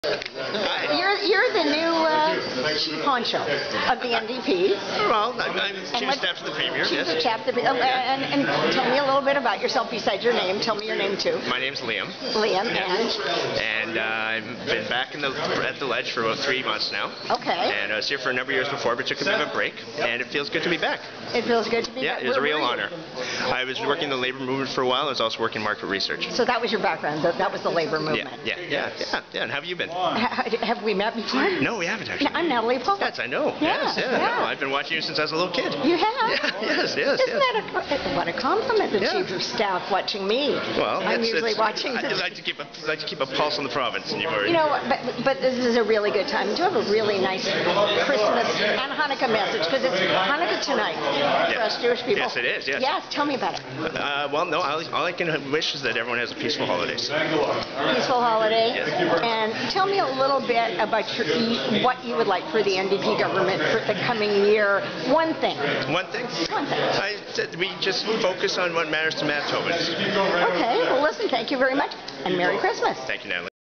Liam Martin; Manitoba NDP Premier Greg Selinger's New Chief Of Staff... Son Of MP Pat Martin
Manitoba Premier Greg Selinger's New Chief of Staff and head of the Executive Council is the son of Manitoba Member of Parliament Pat Martin.
Liam Replaces Michael Balagus who was former NDP Premier Gary Doer Chief of Staff for many years.
Liam Martin also worked on NDP election campaigns as an event co-ordinator and in the party's war room.
Martin has served as the communications representative for the Manitoba branch of the Canadian Union of Public Employees.
He was also serves on the the board of directors of the Winnipeg Blue Bombers.